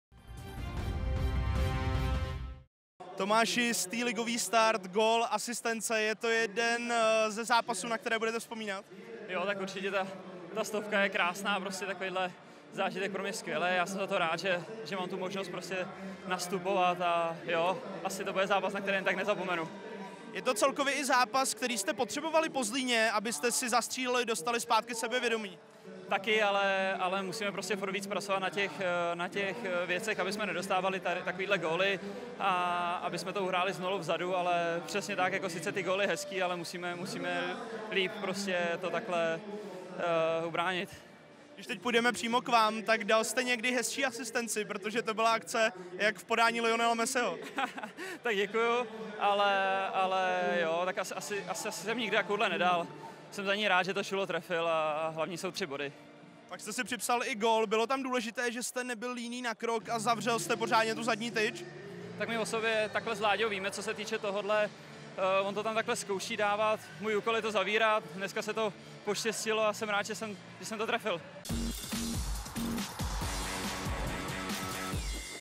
Wiesner po výhře: Zápas, na který jen tak nezapomenu
Tomáš Wiesner oslavil svůj stý ligový zápas za Spartu asistencí a gólem, kterým přispěl k výhře 5:2 nad Pardubicemi! „Stovka je krásná. Já jsem rád, že mám tu možnost nastupovat. Bude to zápas, na který jen tak nezapomenu,“ usmíval se.